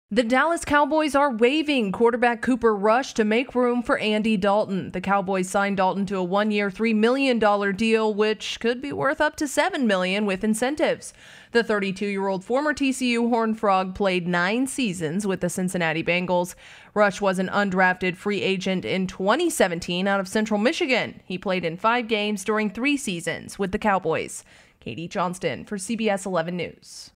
Dallas Cowboys Release QB Cooper Rush As Andy Dalton Takes Reins As Backup
The Dallas Cowboys signed quarterback Andy Dalton on Monday and waived Cooper Rush, who had been the backup to Dak Prescott since the middle of the 2017 season. Katie Johnston reports.